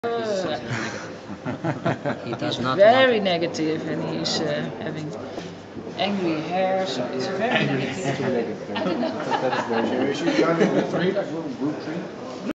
NPSIG workshop
group work
"Tailor-made and weather-proof -- Building and maintaining New Professionals groups as an important part of early career development"
23 January 2013
Ankara, Turkey